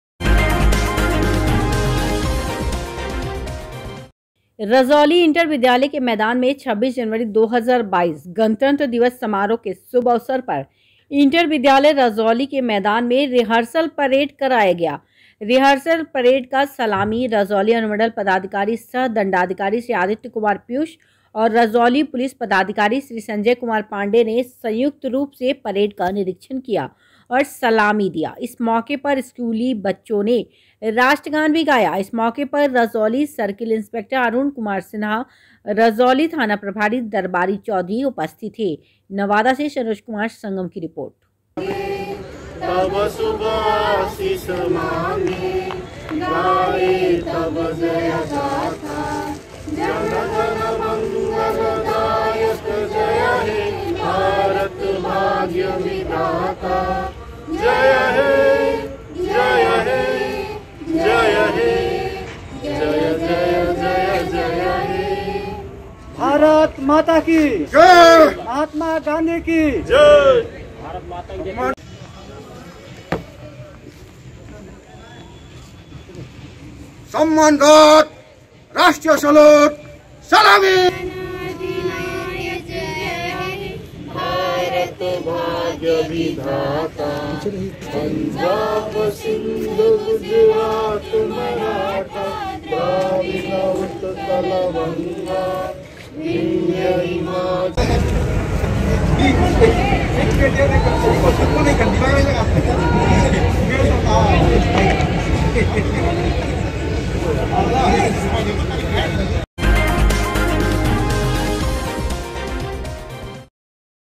26 जनवरी 2022 गणतंत्र दिवस समारोहके शुभअवसर पर इंटरविद्यालय रजौलीके मैदान में रिहर्सल परेड कराया गया
26 जनवरी 2022 गणतंत्र दिवस समारोह के शुभ अवसर पर इंटर विद्यालय रजौली के मैदान में रिहर्सल परेड कराया गया